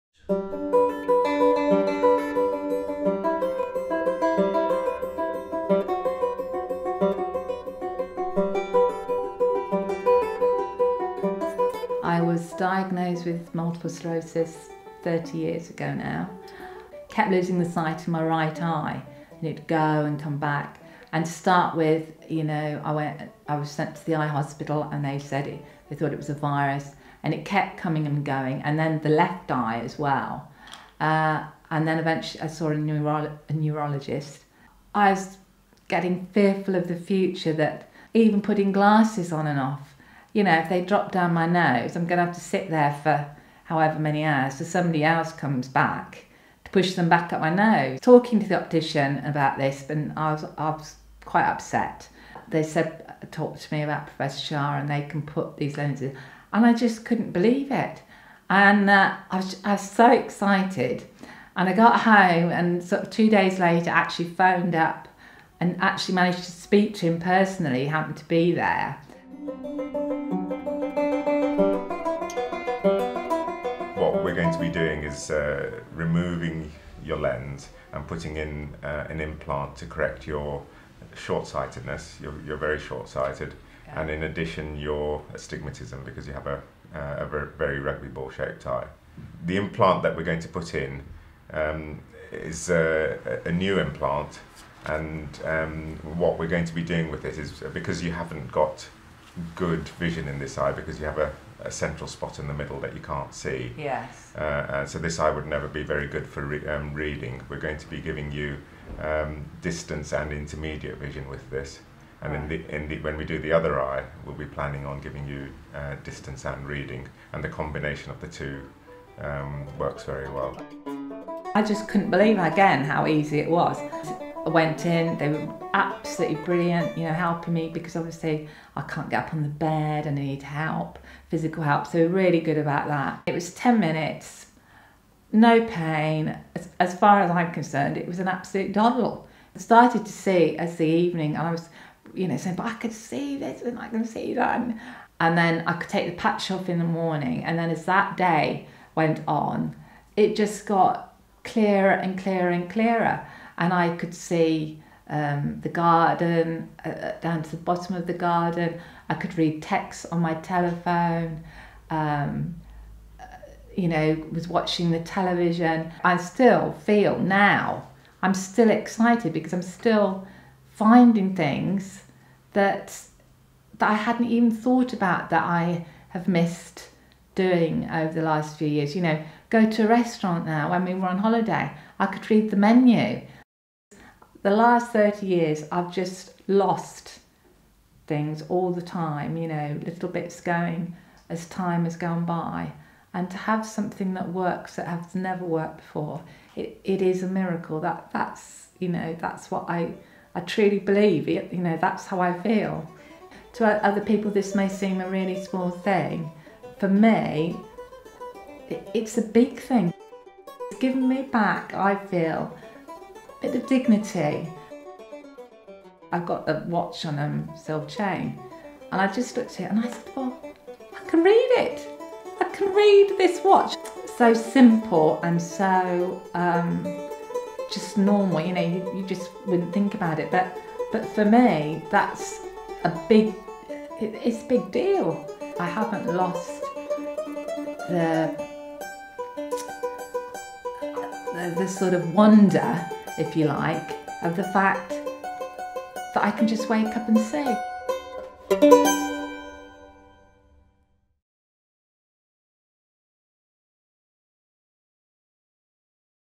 Trải nghiệm của Bệnh nhân sau Phẫu thuật đục thủy tinh thể với Lentis Comfort Toric
- Bà được chẩn đoán là bị bệnh đa xơ cứng, mất thị lực mắt phải cách nay nhiều năm. Bà đi khám ở bệnh viện và bác sĩ cho rằng có thể là do virus, cứ thể bệnh càng tiến triển tiếp ở mắt trái còn lại.
-Thậm chí sau cùng, bà đã đến gặp một nhà thần kinh học,và rất sợ khi nghĩ đến tương lai ngay cả đeo kính cũng o thể cải thiện được thị lực.
- Điều may mắn cuối cùng có người khuyên bà đến gặp bác sĩ nhãn khoa chuyên ngành và bác sĩ đã phẫu thuật chỉ với 10 phút, không đau đớn, thị lực ngày càng được cải thiện rõ ràng hơn.Bà có thể nhìn thấy mọi vật xung
quanh thật rõ nét. Có thể nhìn điện thoại, xem ti vi, nhìn thực đơn khi vào nhà hàng... những sự vật mà bà đã không nhìn thấy cách đây vài chục năm. Và đó dường như là một phép lạ may mắn xảy đến với bà.